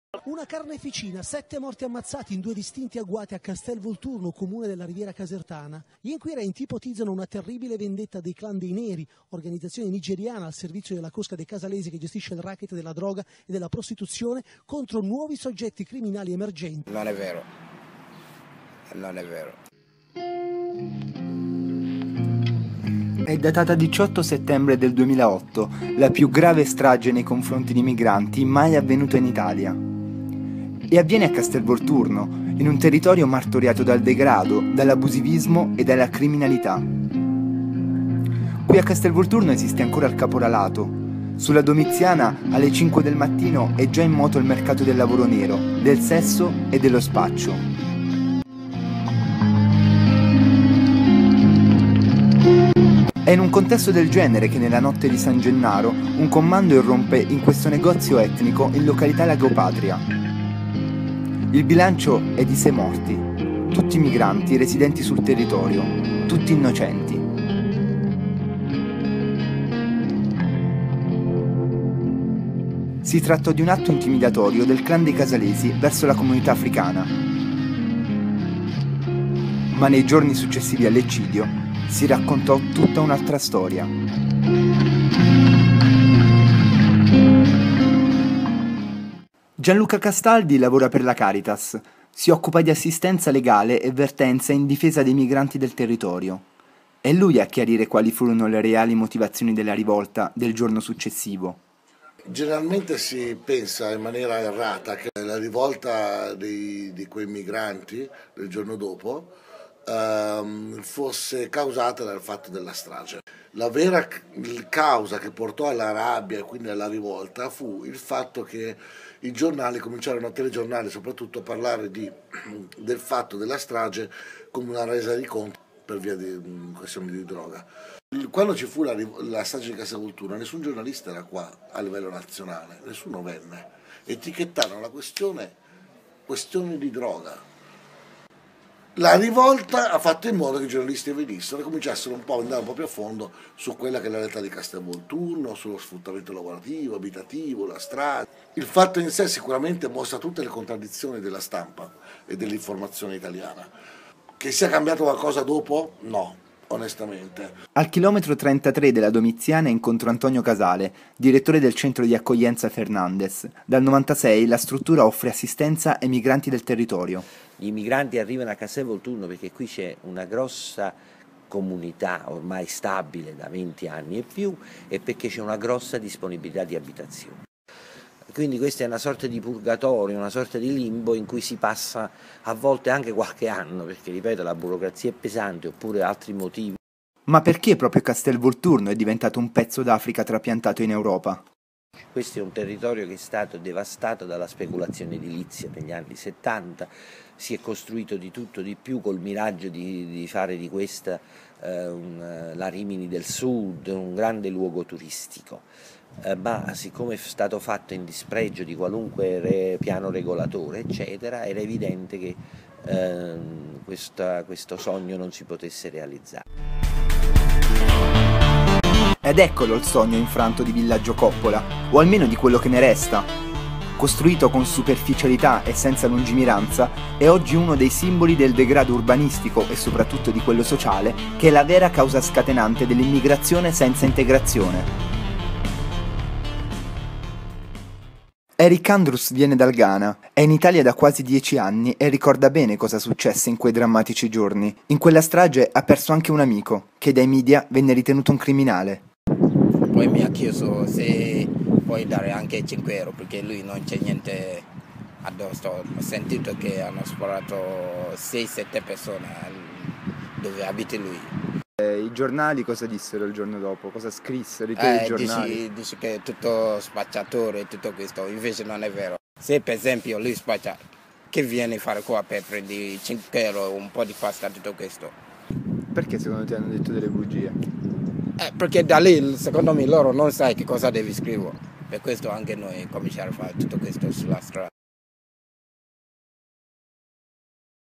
LA RAPPRESENTAZIONE MEDIATICA DELLA STRAGE DI CASTEL VOLTURNO - di Antonio Borrelli
LA RAPPRESENTAZIONE MEDIATICA DELLA STRAGE DI CASTEL VOLTURNO
Video reportage realizzato nel marzo 2015 da Antonio Borrelli in allegato alla Tesi di Laurea in Mediazione Linguistica e Culturale all'Università degli Studi di Napoli "l'Orientale". Il video è stato proiettato il 18 aprile 2015 in occasione della discussione della Tesi di Laurea in Antropologia Culturale sulla "Rappresentazione Mediatica della Strage di Castel Volturno", relatrice prof.ssa Flavia Cuturi.